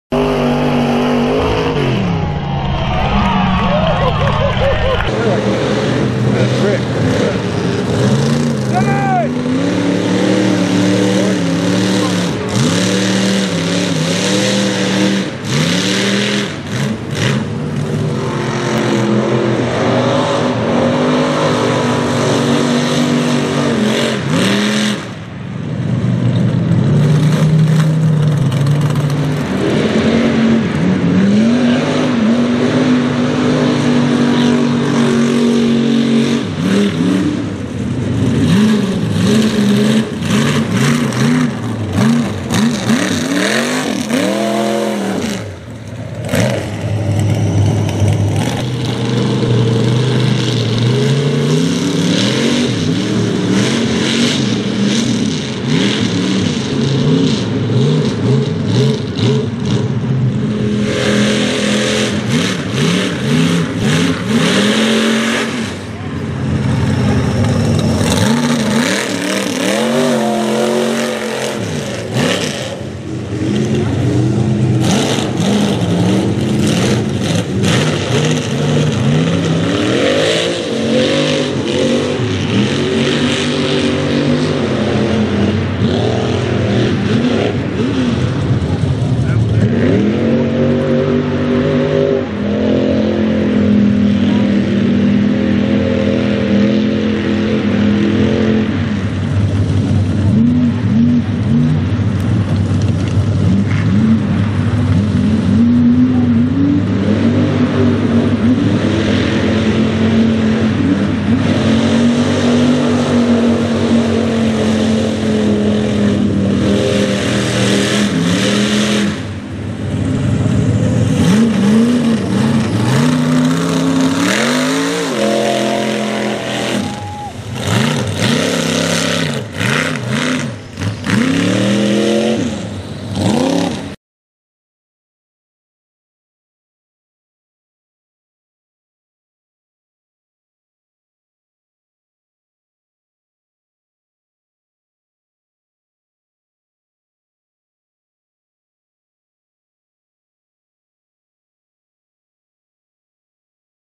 BFE MUD BOG - MIDNIGHT EXPRESS Chevy Mega Truck
MIDNIGHT EXPRESS Chevy Mega Truck At BFE Mud Bog Fall 2019

Footage from the BFE Fall Mud Bog 2019 - All the mud and mega truck mudding action from this mud Bog. Located in Michigan. Playing in some deep mud and sloppy muck.